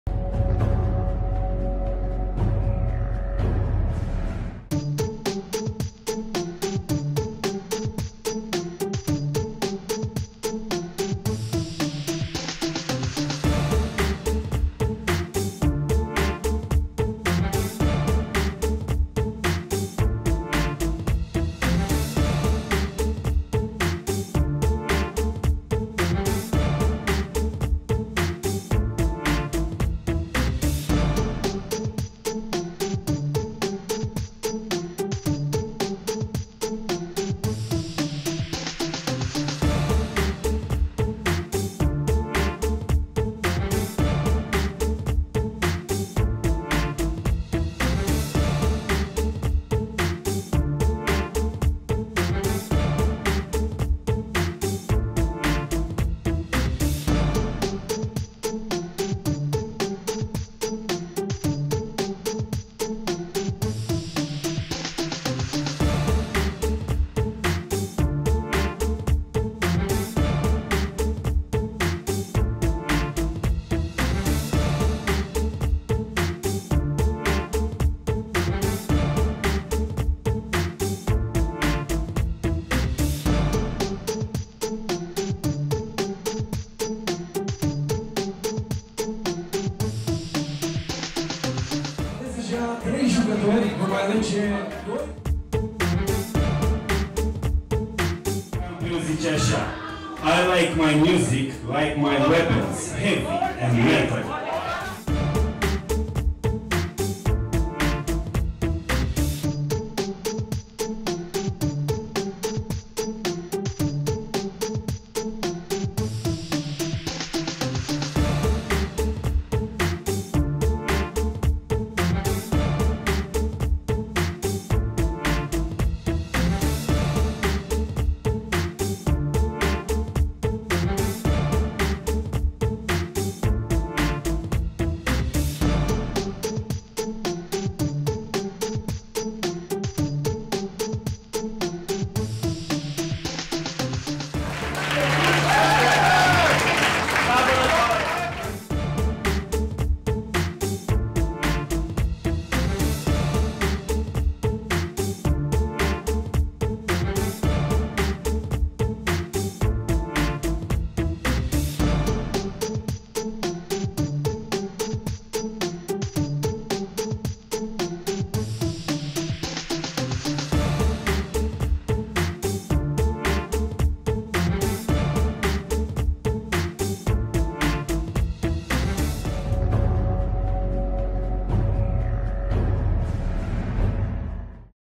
Summoners Gathering 2 - Shadow Isles @ Nexus Bucharest
Dovlecii de Halloween au pazit a doua editie a Summoner's Gathering, care a umplut Nexus Pub & Bar.